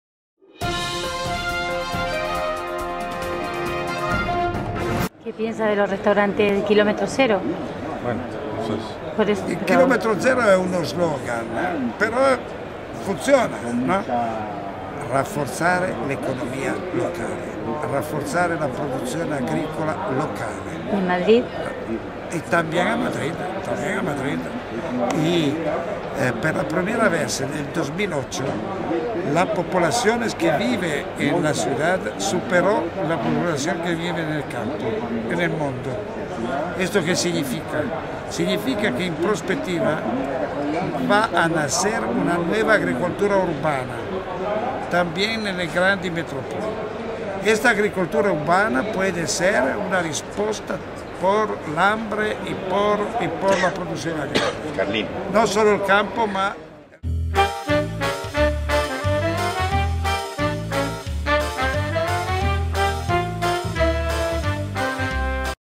Slow Food con Carlo Petrini y La Nueva Agricultura Urbana
Carlo Petrini habla sin pelos en la lengua!! Y nos cuenta que deberíamos hacer y hacia donde vamos!! Slow Food cada día con mas seguidores y sabemos porque!